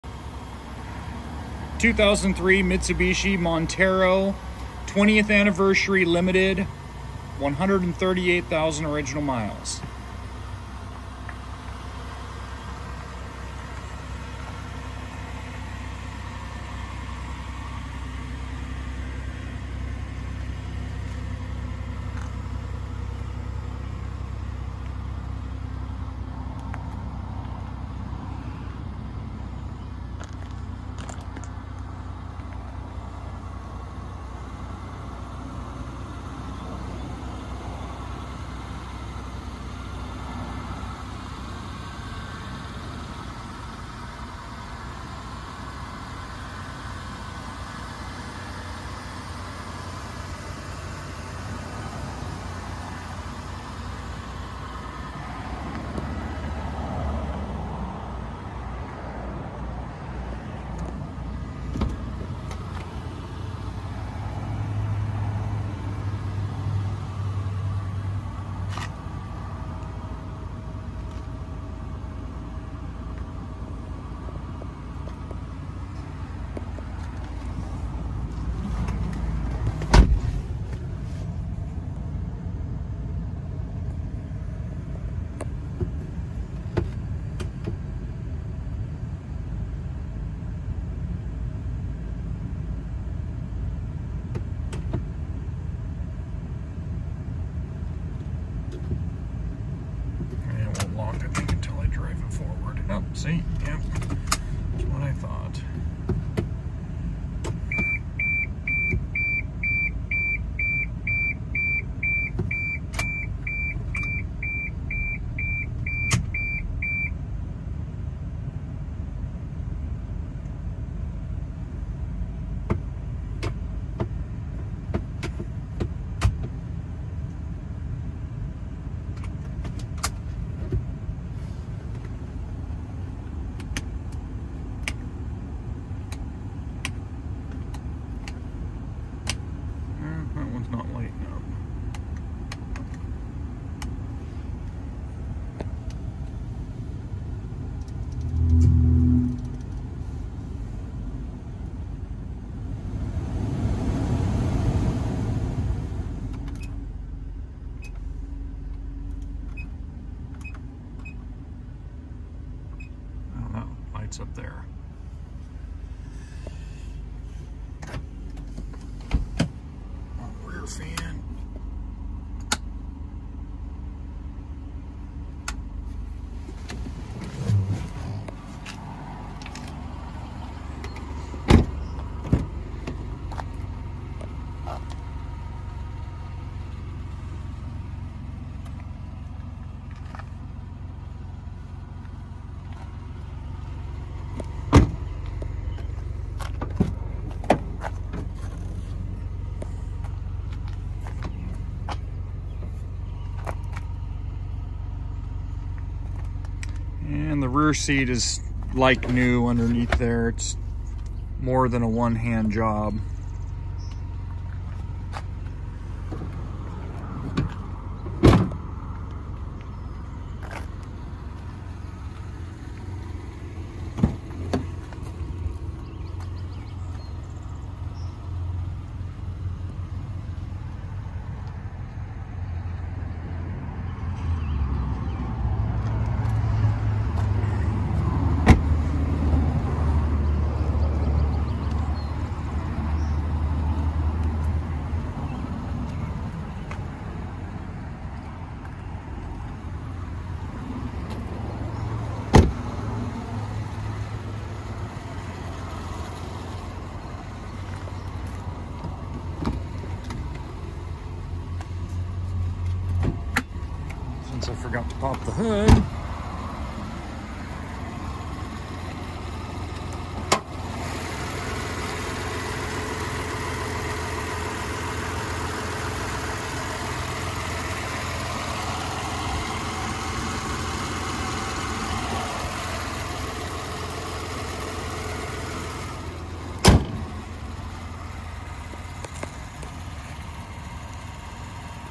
2003 Mitsubishi Montero 20th anniversary 4x4 138,000 miles SOLD ( For Sale)@hallmotorsllc5155
2003 Mitsubishi Montero Limited 20th anniversary 4X4 only 138,000 miles. Full WALKAROUND with the vehicle running.